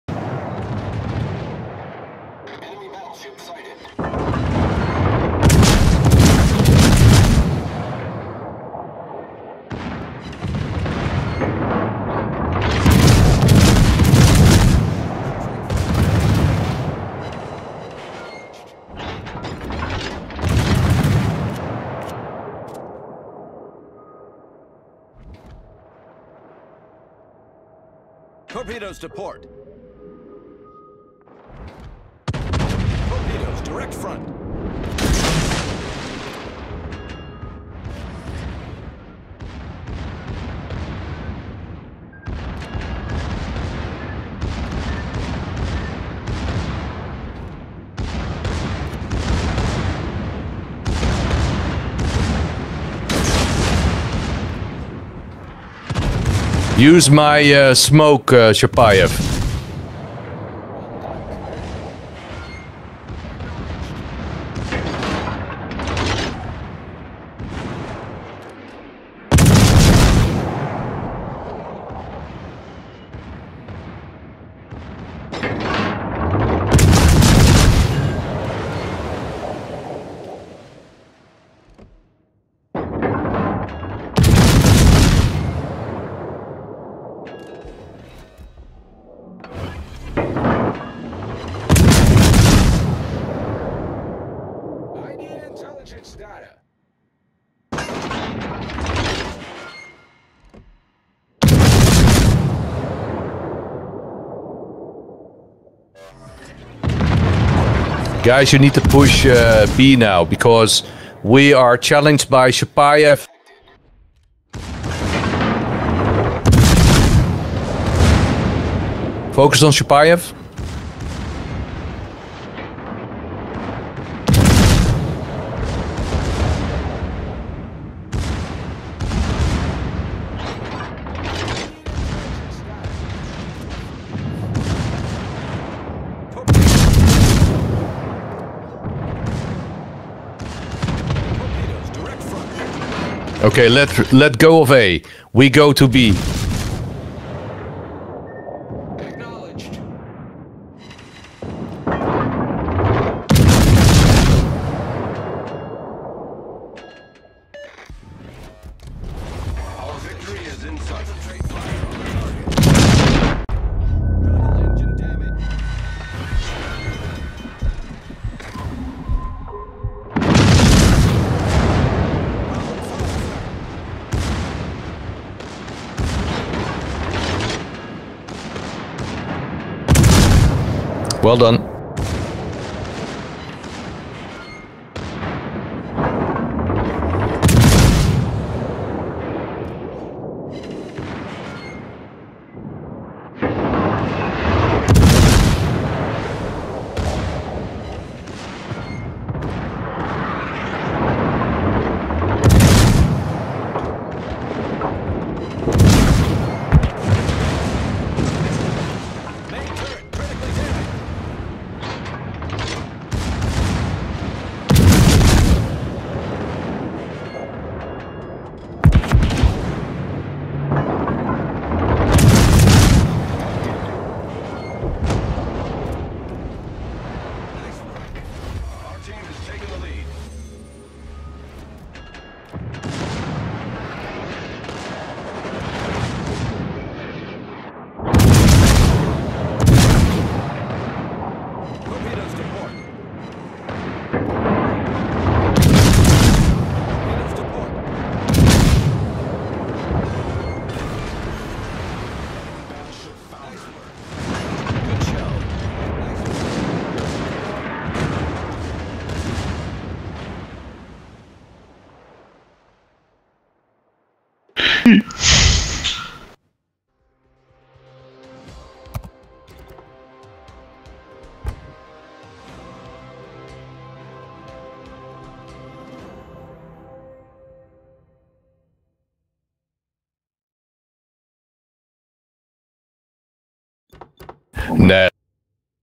Clan Battles T8 Battle Zone Hell
One of the most fiercing battles I ever played in CB.-